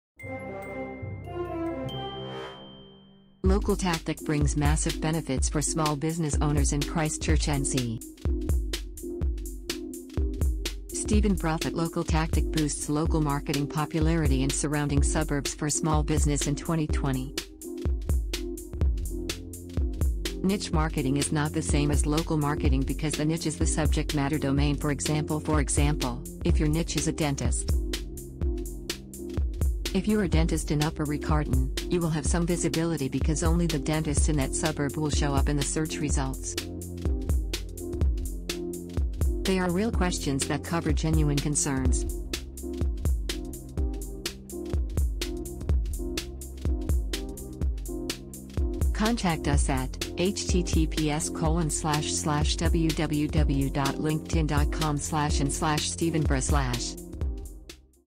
LOCALTACTIC Brings Massive Benefits for Small Business Owners in Christchurch NZ
LOCALTACTIC brings the massive benefits of local marketing to Christchurch for small businesses by boosting popularity in surrounding suburbs. We specialise in local marketing. We believe large amounts of wealth are stored in local results and it is these results businesses should be working on.